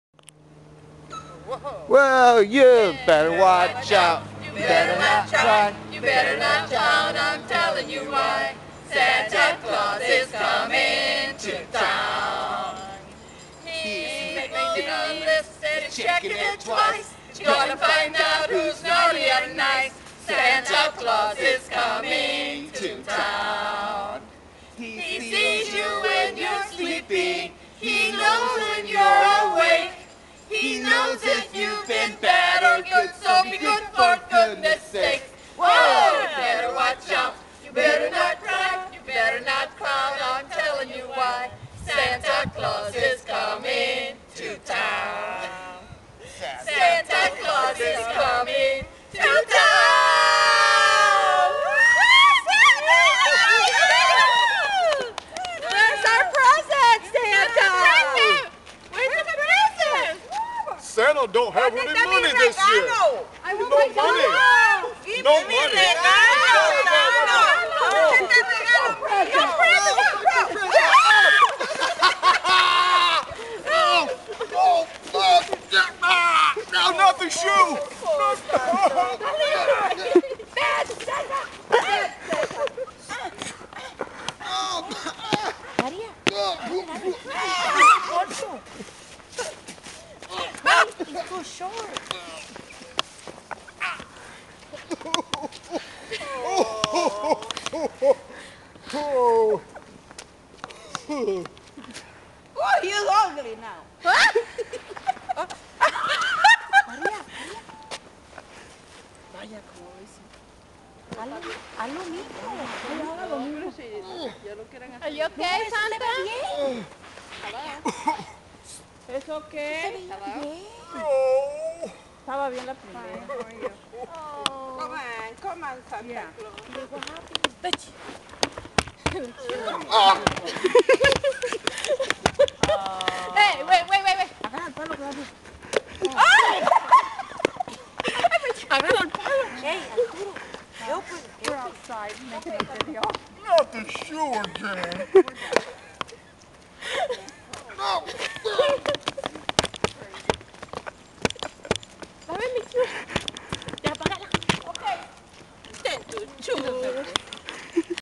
santa gets the shoe
in this bad economy,even santas packets are empty
a new video by andy edelman,claudia gonzalez and a groups of friends maria rivera
,maria salazar,caroline gill and tutty lion